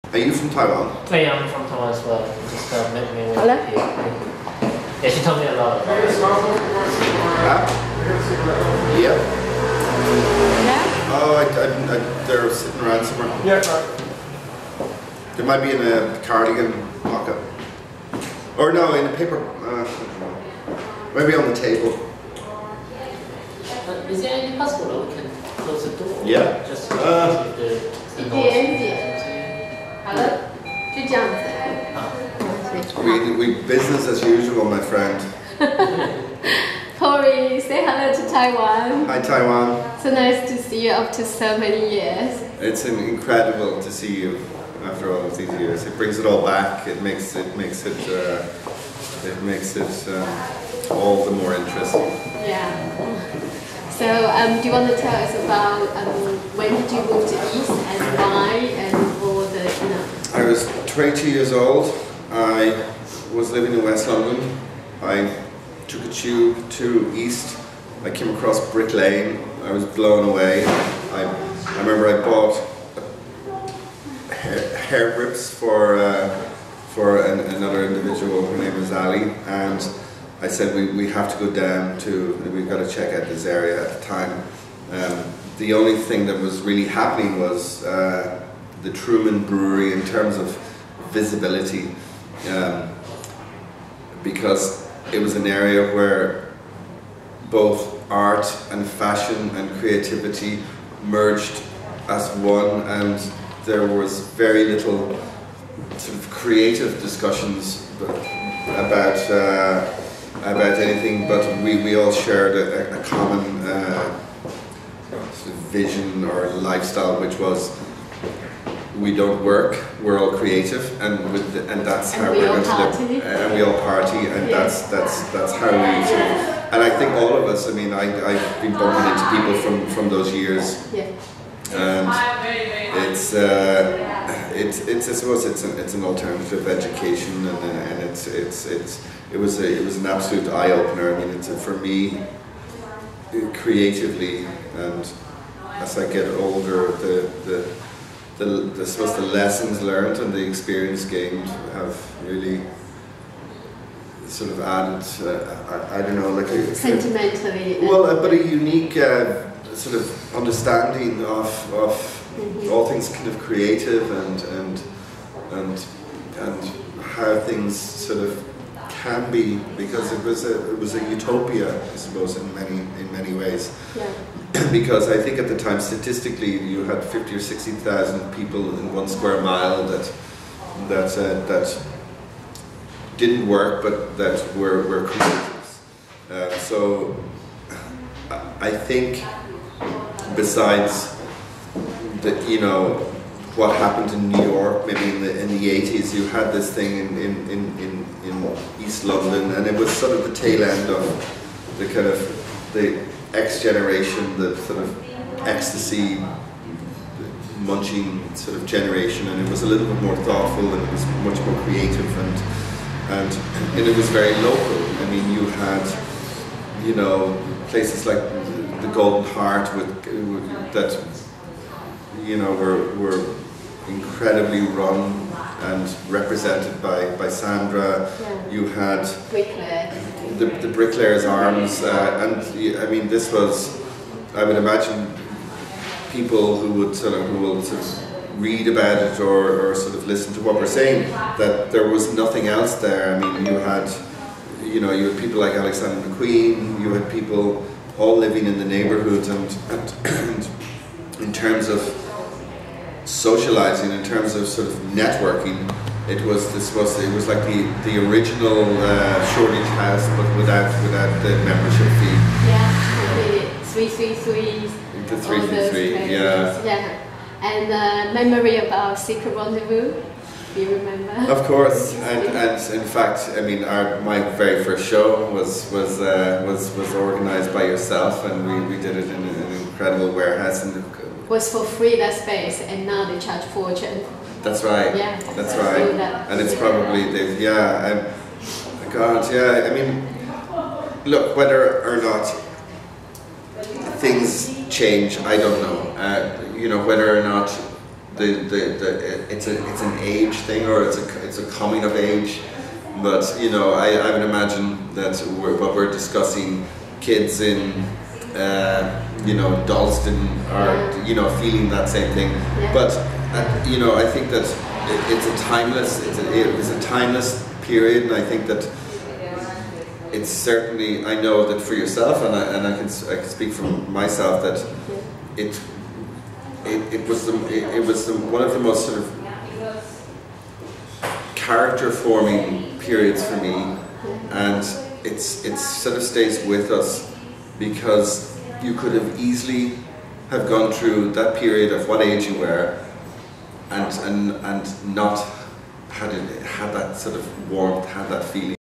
meihui liu interview Pauric Sweeny for Secret rendez-Vous
Visit one of great friend from the old day  Pauric Sweeny Shop in Mayfair London and interview Pauric for Secret Rendez-Vous
Pauric talk about East London back to the old day

08 Nov 2012